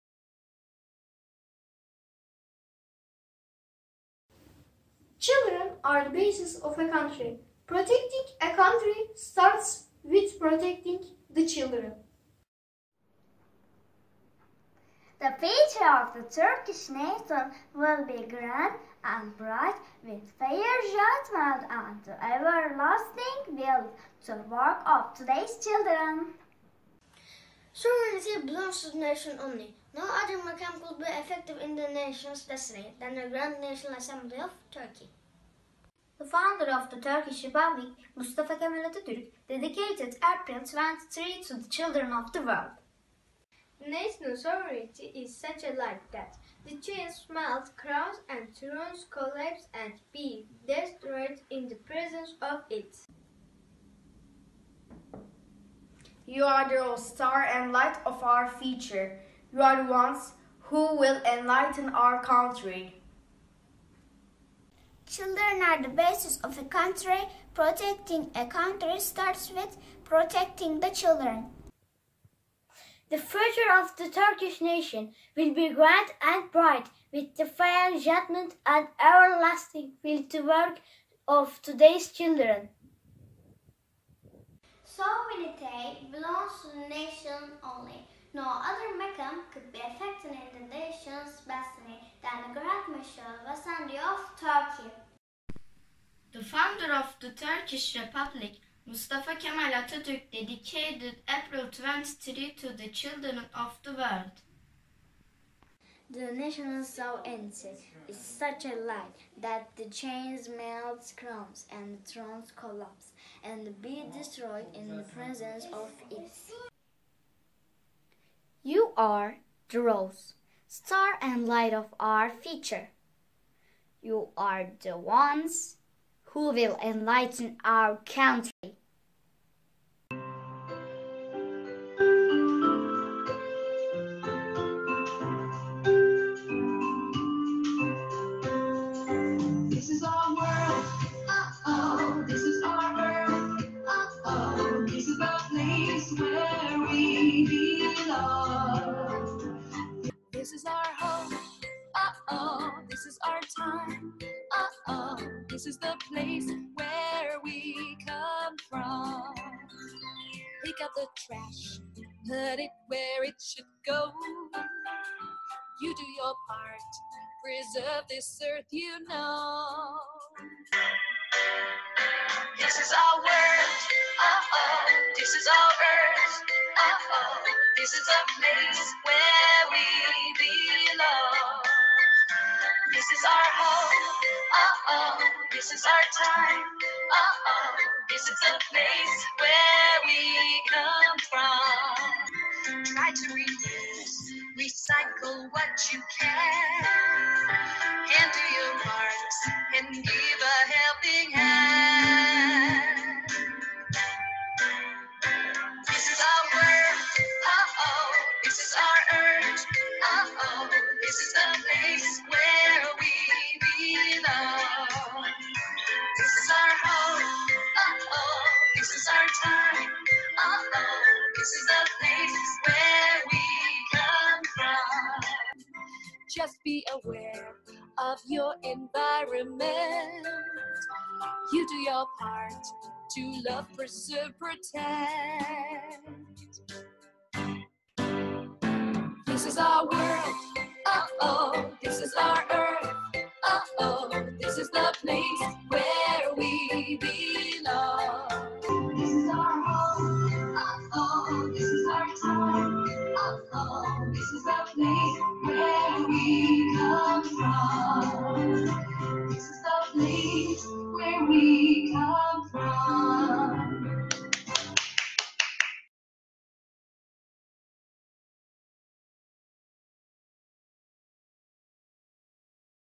23 NİSAN 100. YIL ANISINA
İlkokul 3 ve 4. sınıflarımızın öğrencilerinden 23 Nisan ile ilgili mesajlar... (İngilizce)